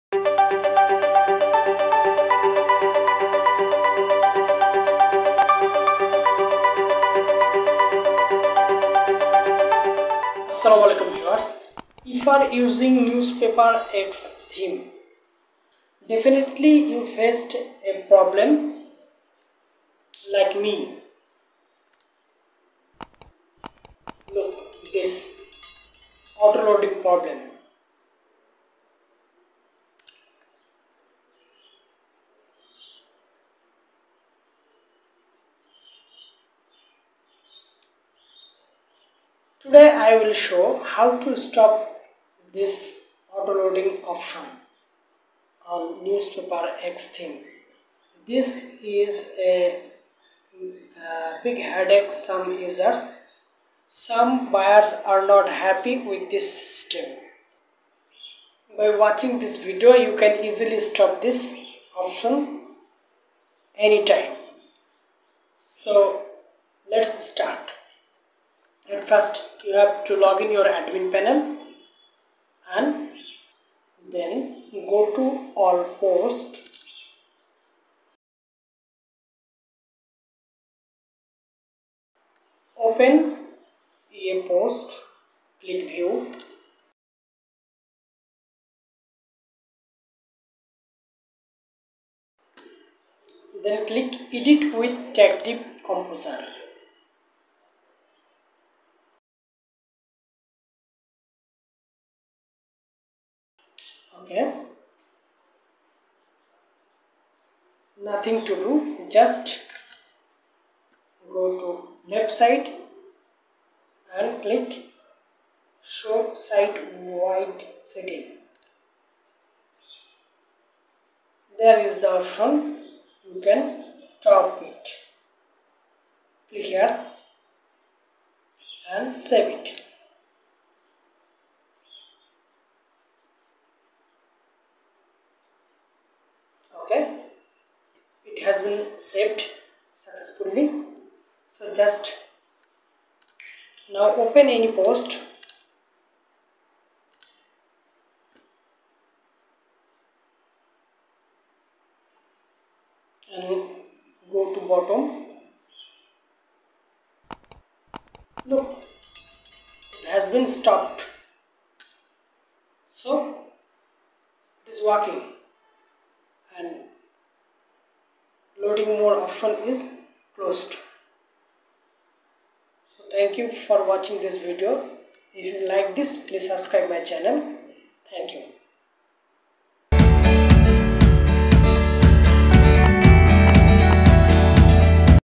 How to stop auto loading system on newspaper x theme
If you are using newspaper x  theme in WordPress you can stop infinity load on the posting page.

w3techbd is provided you all kinds of web services, including web to mobile applications, Video Editing, Creating advertisements, and Content writing.